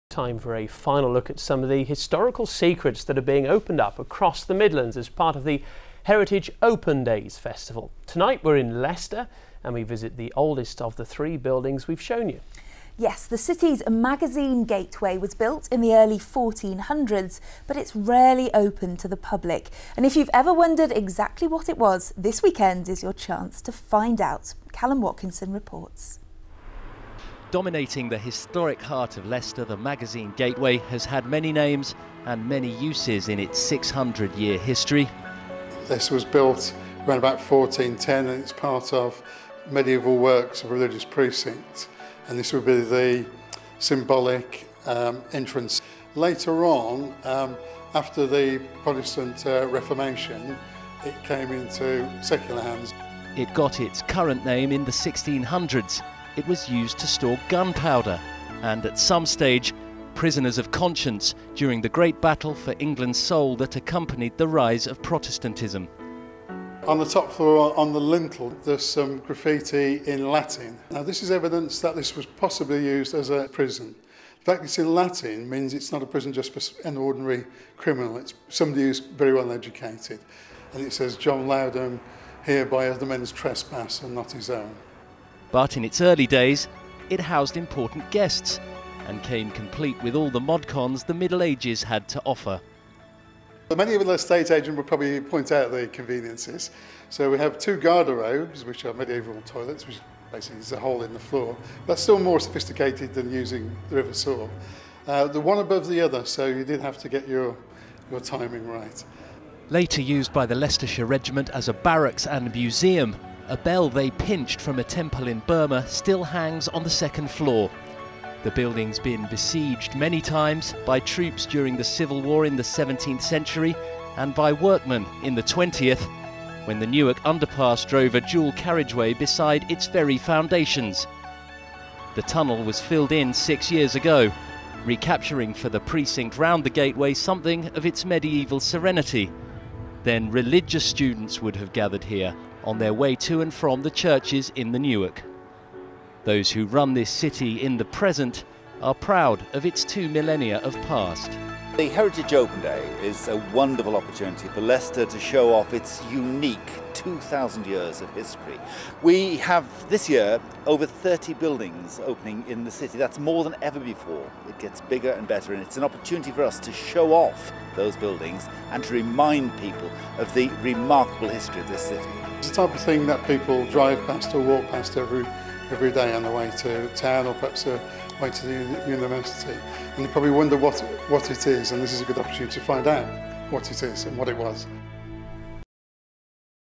HERITAGE OPEN DAYS FEATURE: LEICESTER MAGAZINE GATEWAY (13/09/2013)
A final look at some of the historical secrets that are being opened up across the Midlands as part of the Heritage Open Days festival. Tonight we're in Leicester and we visit the oldest of the three buildings we've shown you. The city's Magazine Gateway was built in the early fourteen hundreds but is rarely open to the public. If you've ever wondered exactly what it was, this weekend is your chance to find out.